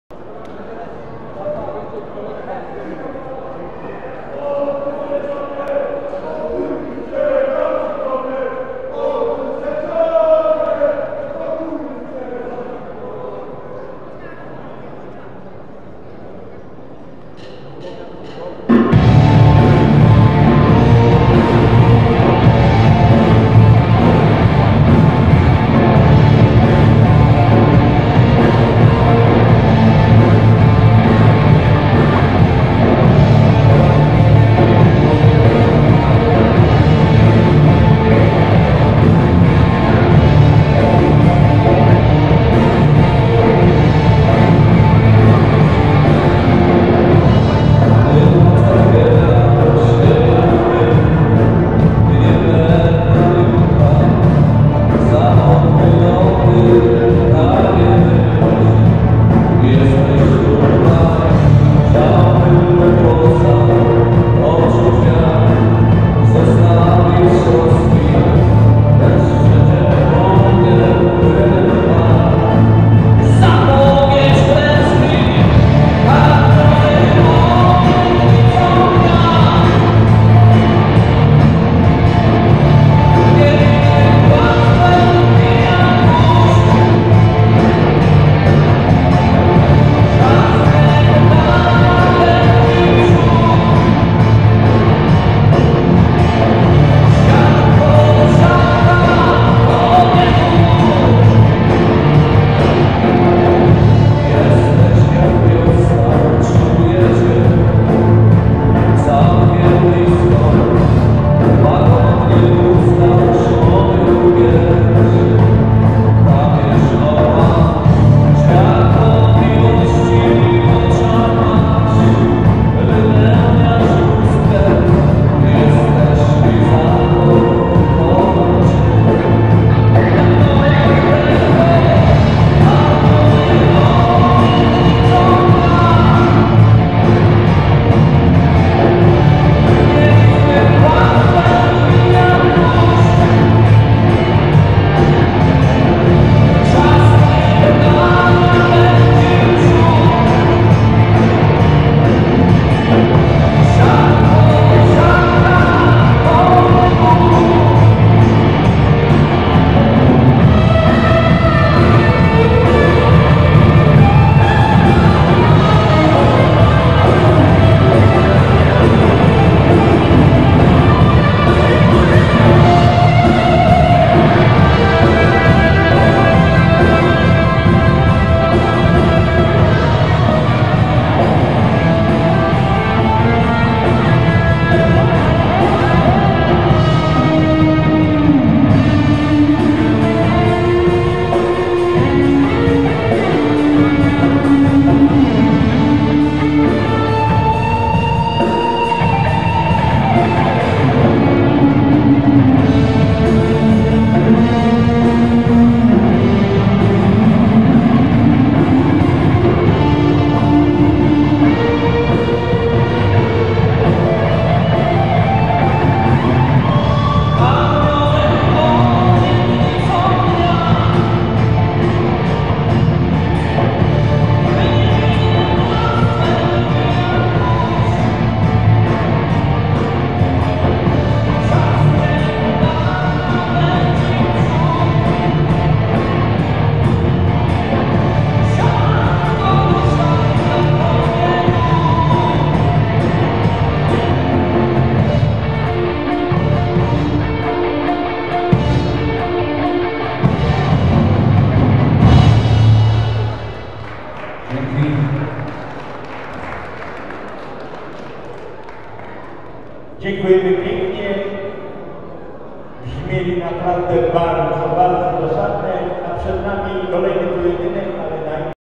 Gala CSW 'Ring Wolny' - zespół ISCARIOTA
- Największy portal bokserski w Polsce przedstawia występ zespołu ISCARIOTA podczas jubileuszowej Gali CSW 'Ring Wolny' - zorganizowanej przez Centrum Sportów Walki 'Fight Boxing' Dąbrowa Górnicza

Przez cały jubileuszowy turniej na zywo grał zespół ISCARIOTA